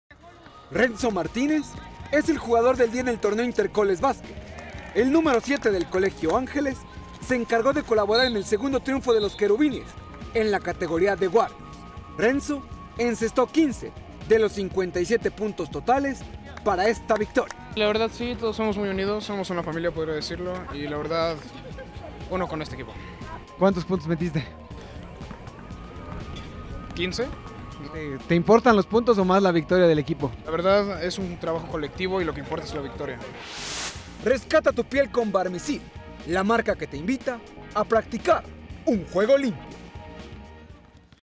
Jugador del día 27/oct/2016
Renzo Martínez fue pieza fundamental en el triunfo de su equipo, realizó 15 anotaciones, por ello se convirtió en el jugador del día.